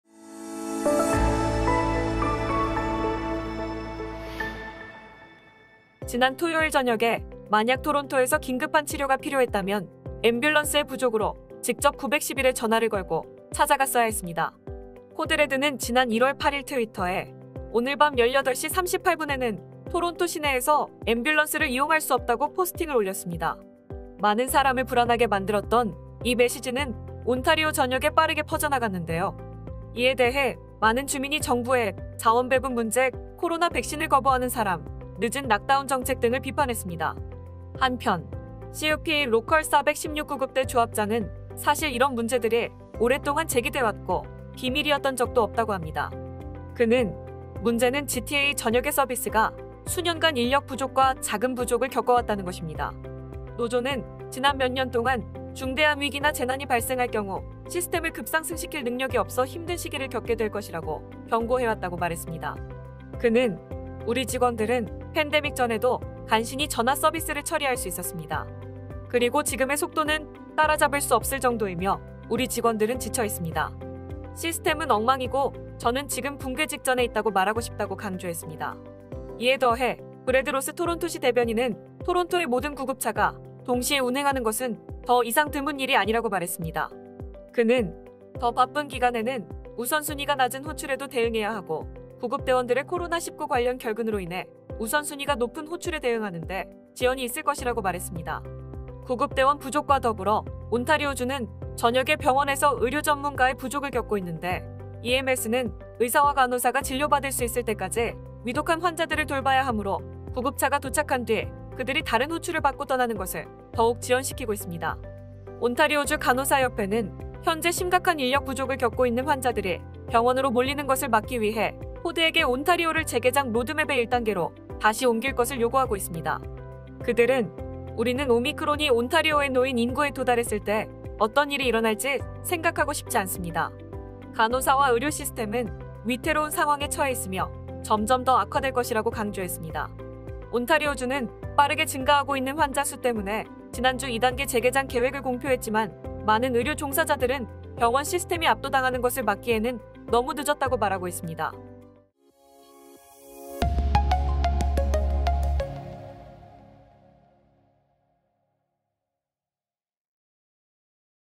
토론토 앰뷸런스 콜 포화 상태, 앞으로 상황은 더욱 악화될 것
모든 기사는 확인하실 수 있습니다.